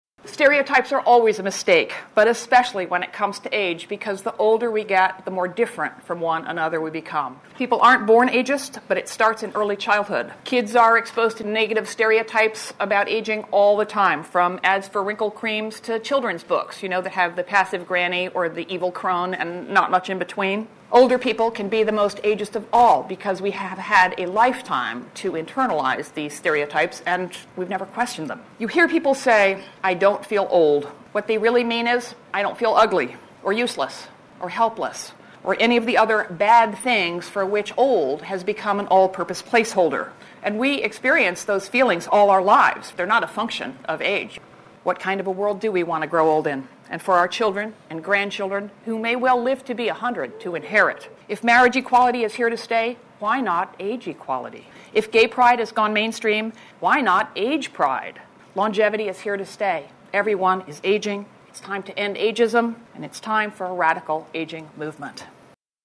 FILM - End Ageism
A brick film stop-motion animation I did while at University.
The brief was provided by the RSA student awards and required an animated film to accompany the audio film they had provided.

This was my first attempt at creating a stop-motion style film.

Software used:
Dragonframe
Illustrator
AfterEffects
Photoshop